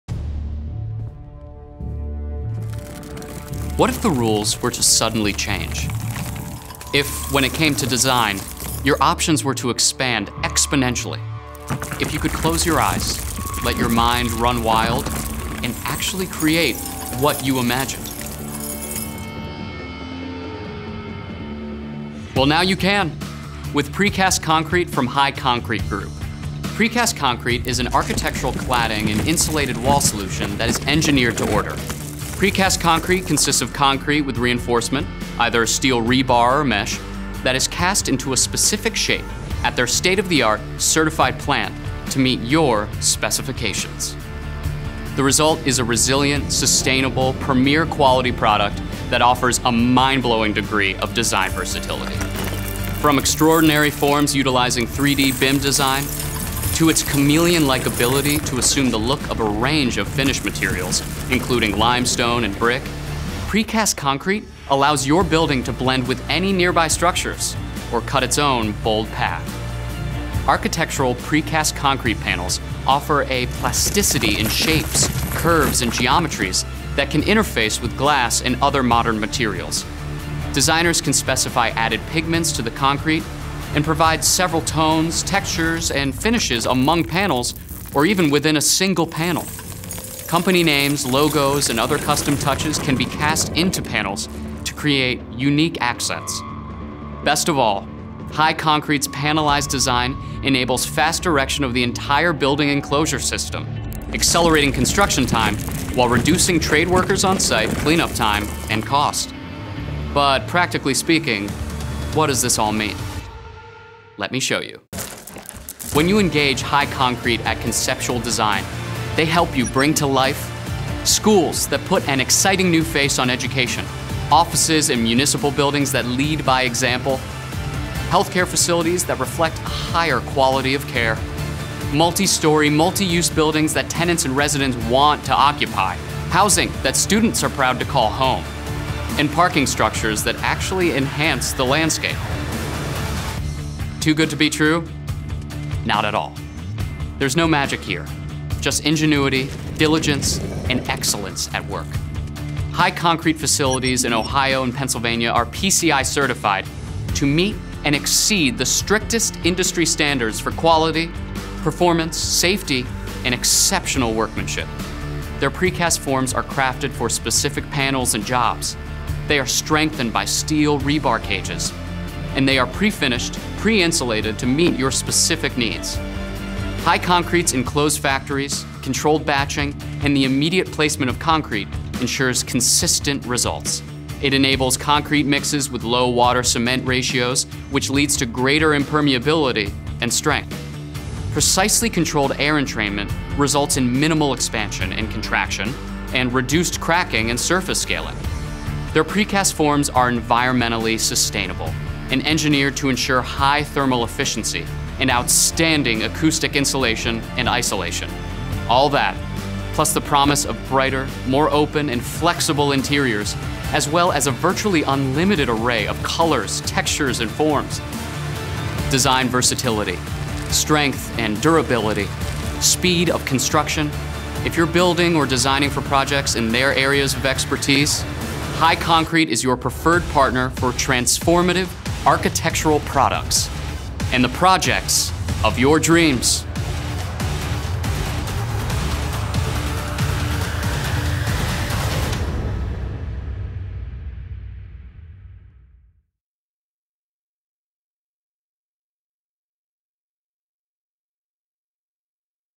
High Concrete Group: Precast 101 (Full length)
Capabilities overview of High Concrete Group LLC, a leading producer of architectural precast and structural precast concrete based in Denver, Pennsylvania.